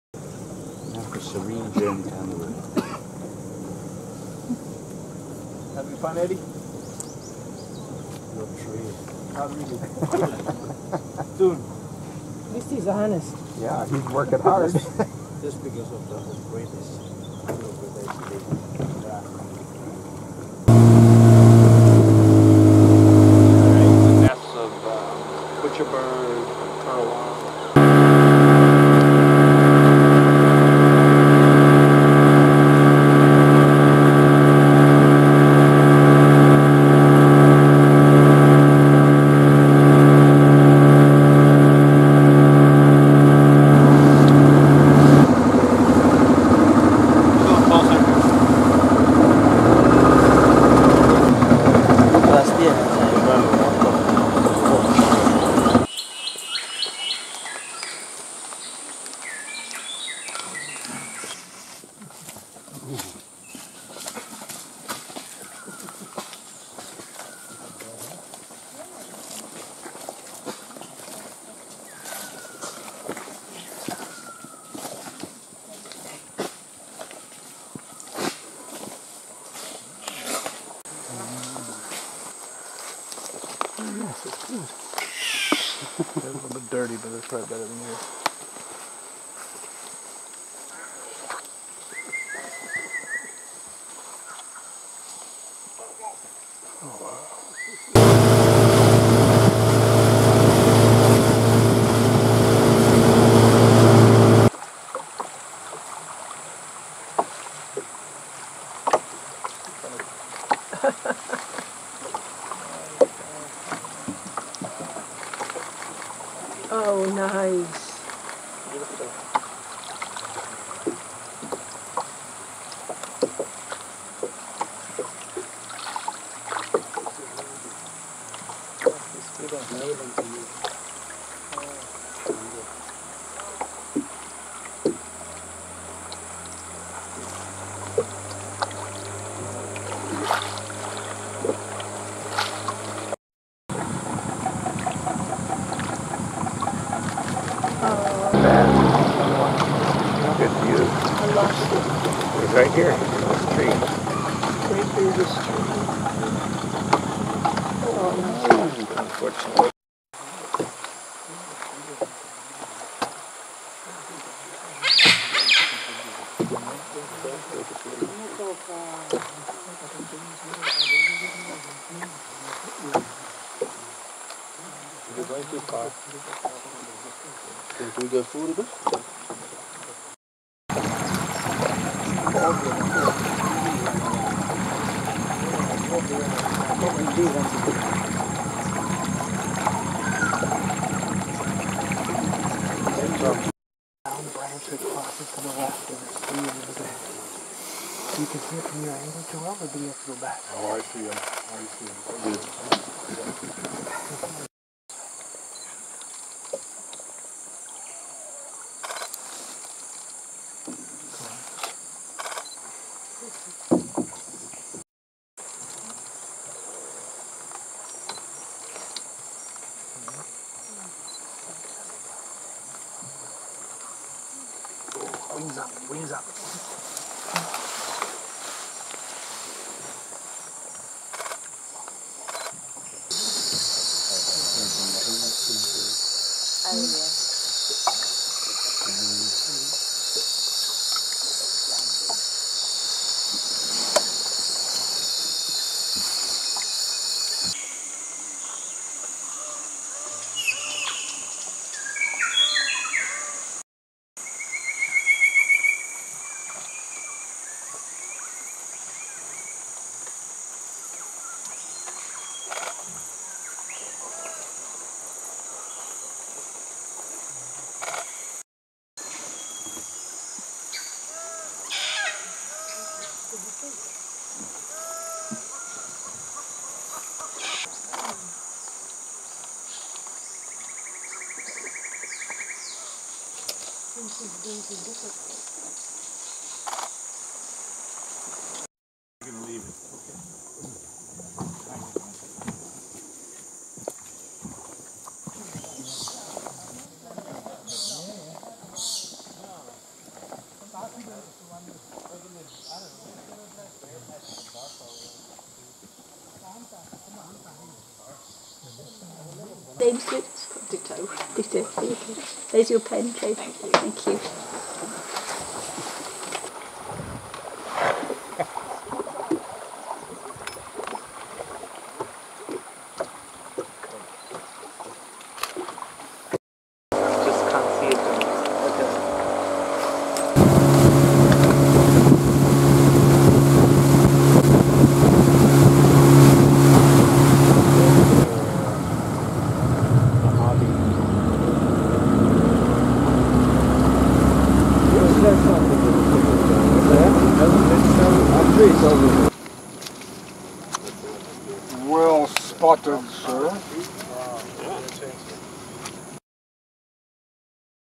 Kwatu / Fly River
Highlights of Kwatu Lodge and the Fly River on Rockjumper's PNG II tour of 2018